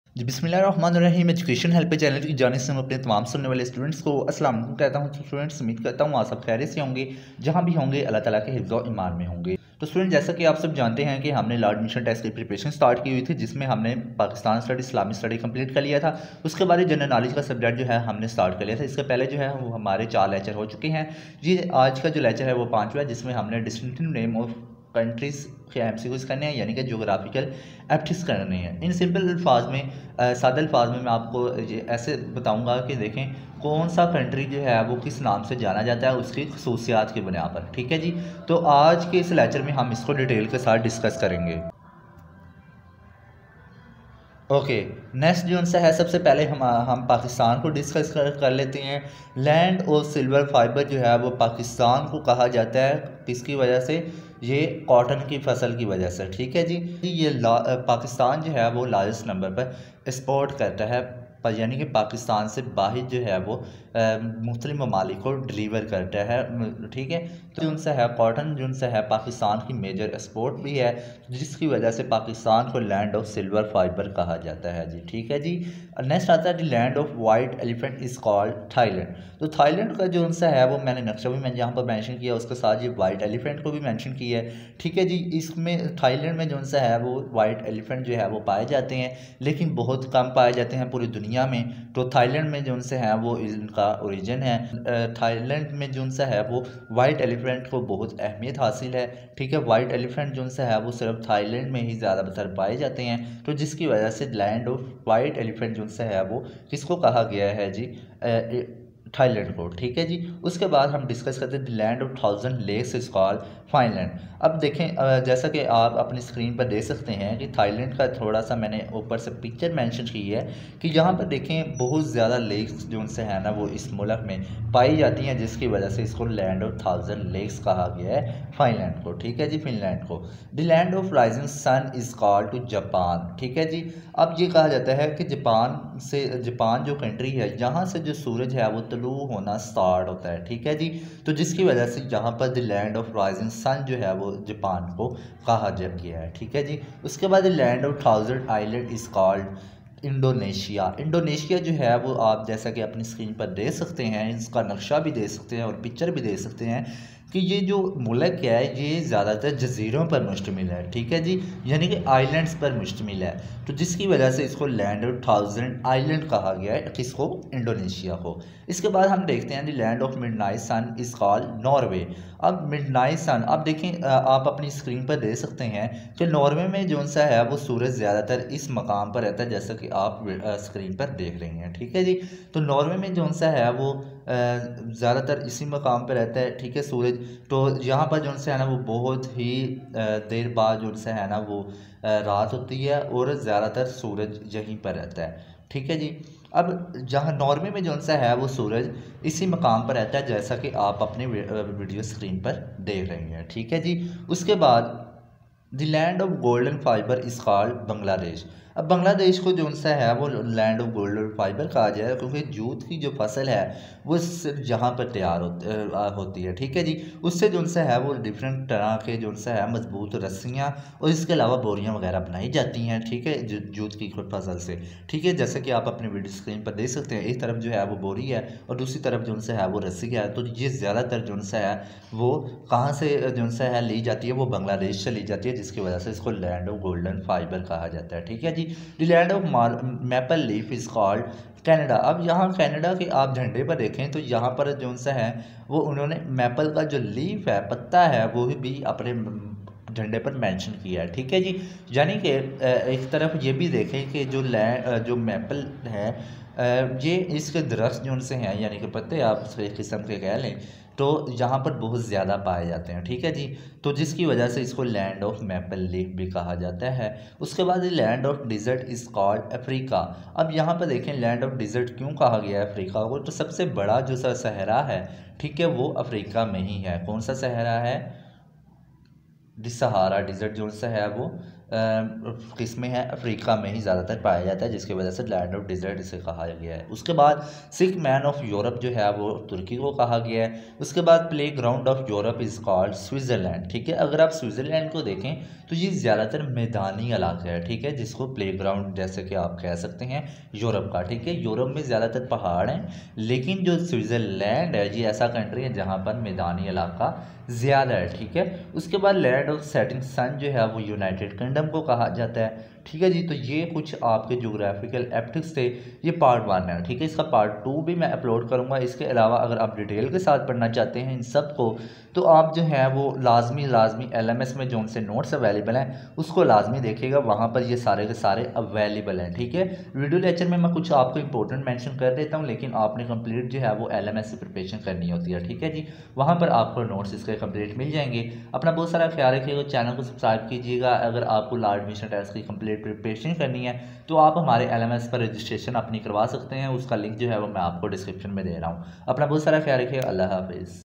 General Knowledge MCQs for LAT | Law admission test preparation | GK
Law admission test preparation general Knowledge MCQs for LAT geographical epithets.